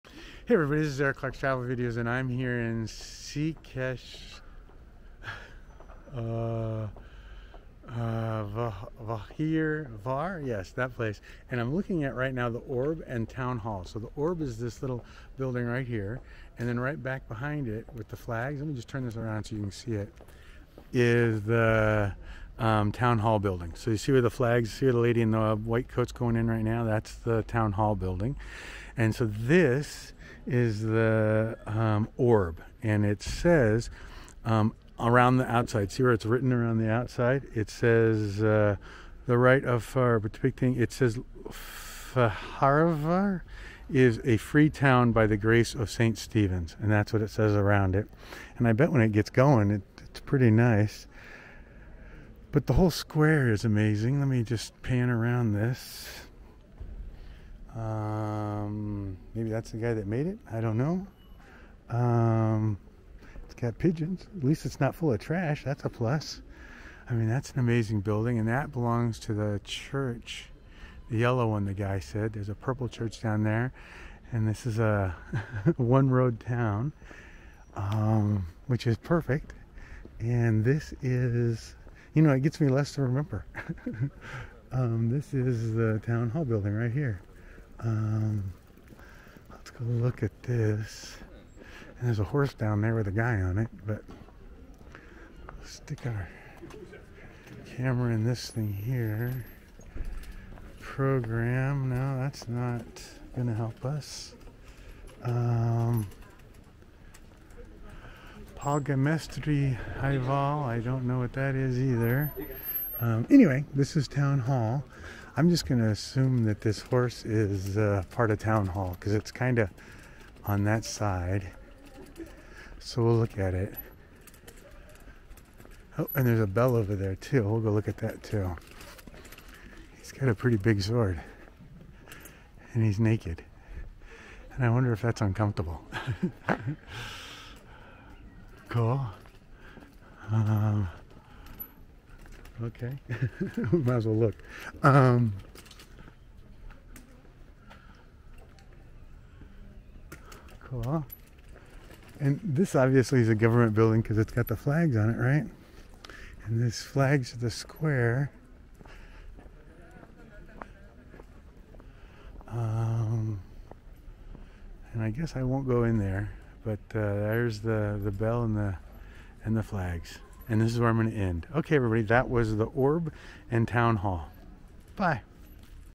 Orb and City Hall, given its free royal town status by St. Stephen. - Székesfehérvár Hungary - ECTV
Székesfehérvár was given its free royal town status by St. Stephen, founder of the state. The work of art- ‘The right of Fehérvár’, in common use it is called the Orb- commemorates this ancient right. The Soviet army boarded it up in 1945. In the 1950s it was cut in half and the upper half was removed with the cross. It regained its spherical shape in the 1960s and in 1986 the cross was also taken back. Three lions hold the Orb above the well-fountain. Each of them guarding a coat of arms of the country, the city and  Andrew II. The three dates below them indicate three important events in the life of the town.

1001 – the coronation of King  St. Stephen I.

1688 – the liberation of the town from the Ottoman rule

1938 – the 900th anniversary of the death of King St. Stephen I.

The Orb is surrounded by the Bishop’s Palace and the Székesfehérvár Diocese Museum but besides them the other characteristic building of the square is the Town Hall. The right wing of the building was recorded as the town hall in the land registry as early as 1698. It holds one of three murals by Vilmos Aba-Novák in the town and also the certified copy of the Holy Crown bought by public donation.